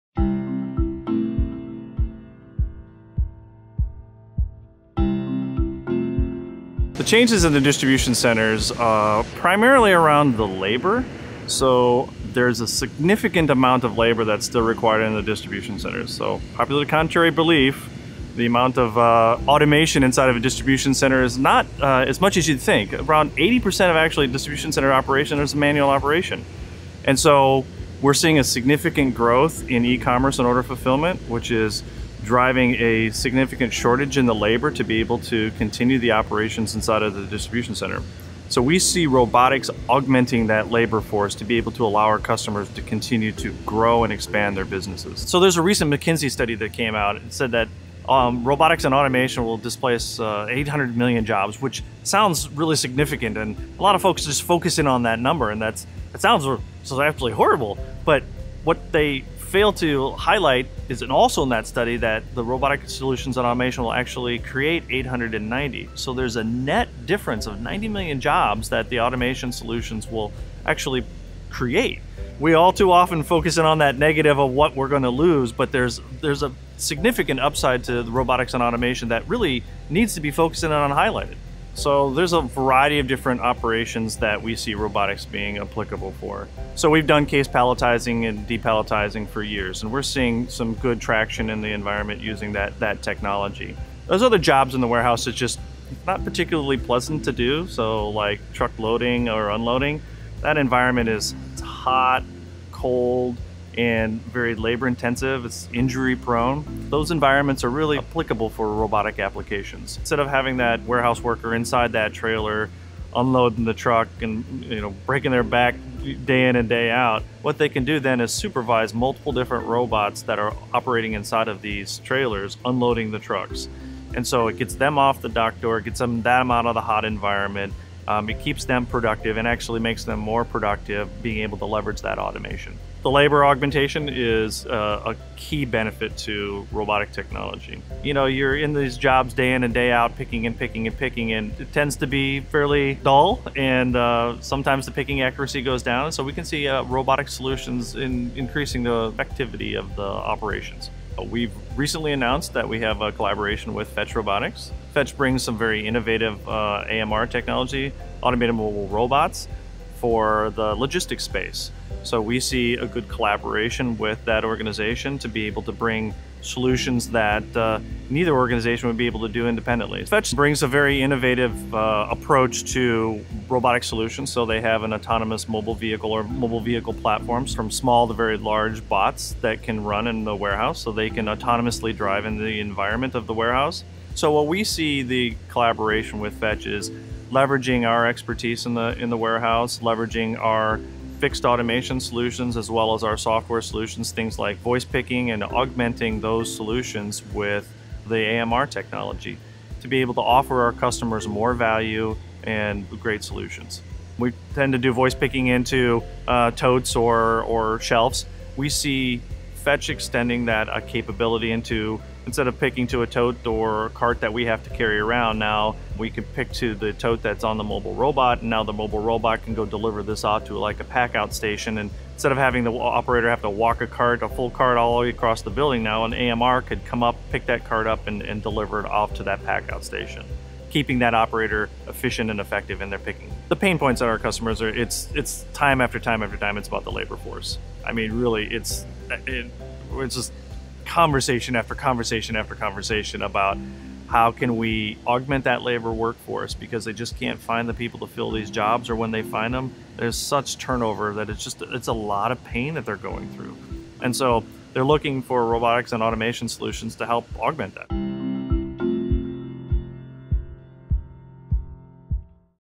Robotics and Labor Force Augmentation
Matt Wicks discusses the labor shortage many distribution centers are facing in response to significant e-commerce growth and how robots are helping offset some of these challenges. There’s still a great need for labor within distribution centers, seeing as how around 80 percent of distribution center operations are manual. Labor augmentation is a key benefit to robotic technology, allowing companies to continue to grow and expand their businesses. Robotics are applicable to a variety of areas in a distribution center such as case palletizing and depalletizing, truck loading and unloading, each picking, sorter induction and more.